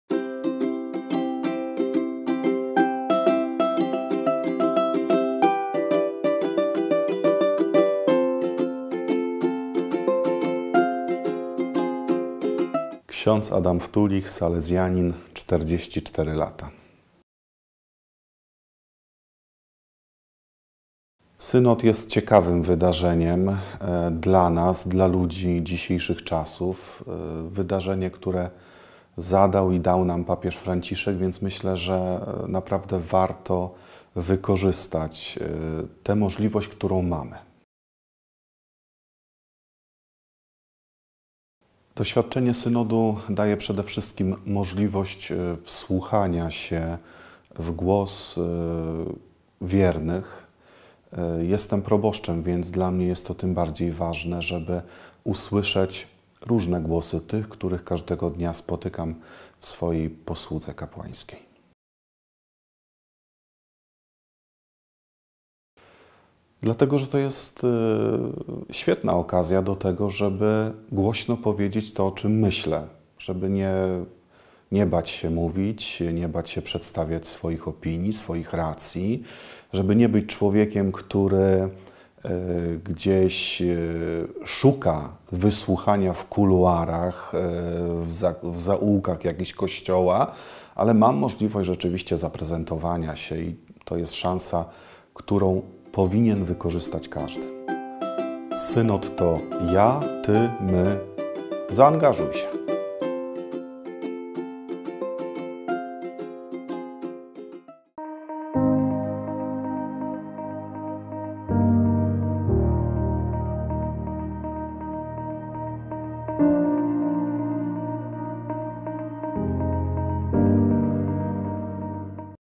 Ks. Adam Wtulich | Synod o Synodzie | Łódź 2022
Spot zachęcający do udziału w etapie diecezjalnym Synodu o Synodzie.